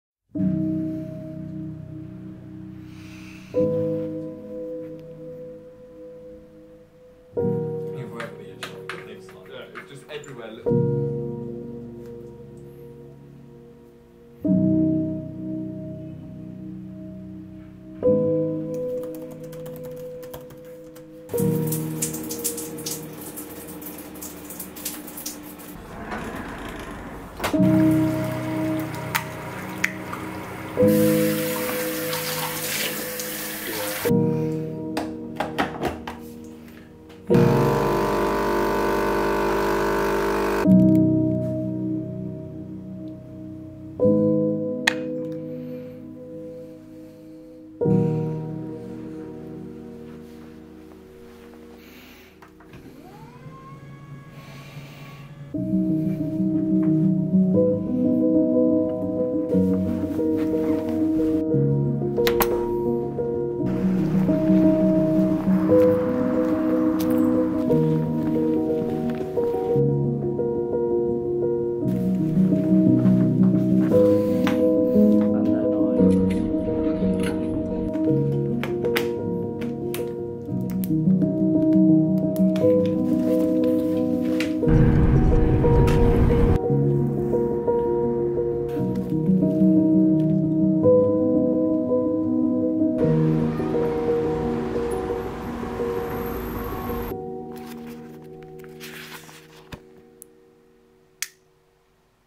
A Mundane Day in 2023 (Short Film)
Inspired by Danny Gevitz. What a normal summer's days looks like for me in 2023.

It was super hard to capture this through me eyes, a normal average day. Much love.